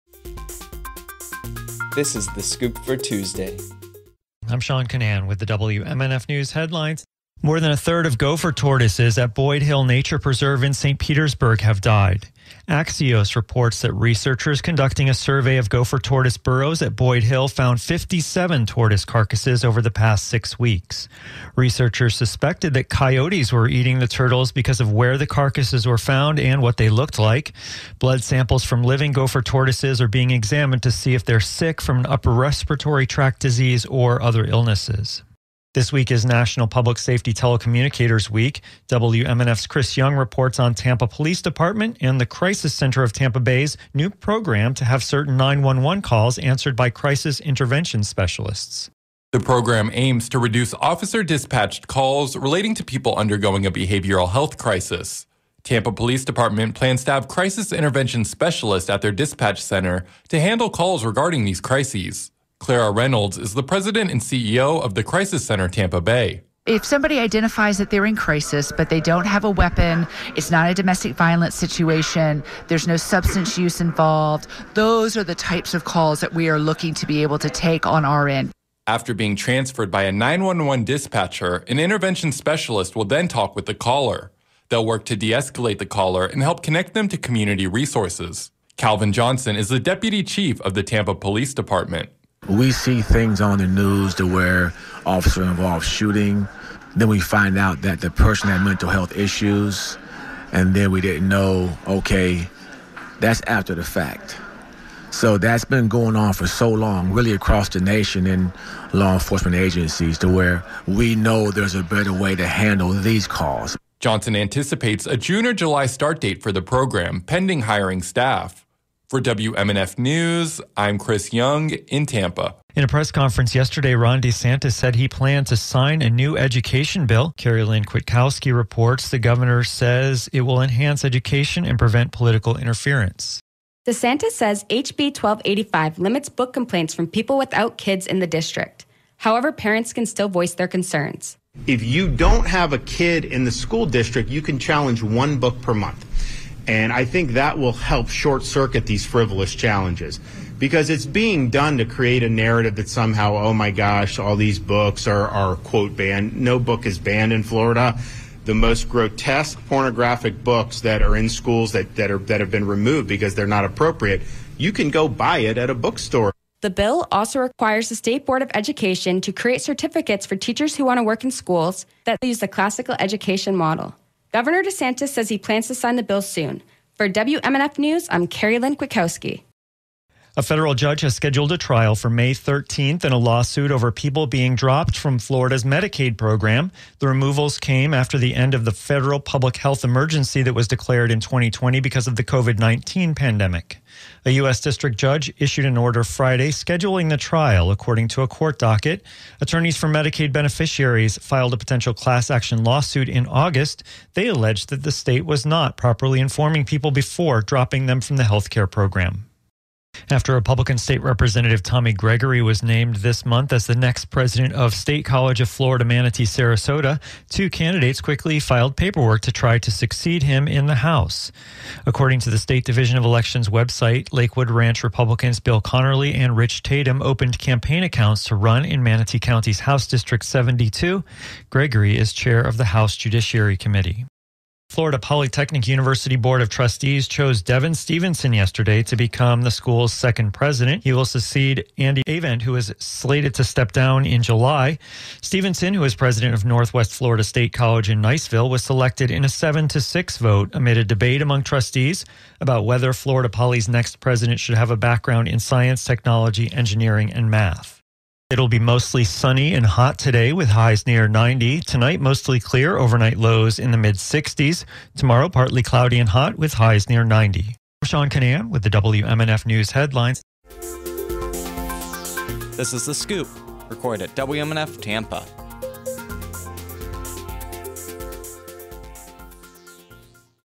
The Scoop: Tues. April 16th, 2024 Tampa Bay and Florida headlines by WMNF
Discussion by college leaders about fostering diversity on campuses, and, join the awareness campaign during Colorectal Cancer Awareness Month.